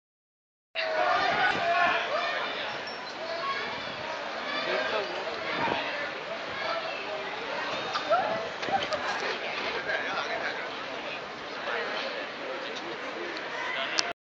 Autographs
One of the stars gets out of vehicle and signs autographs.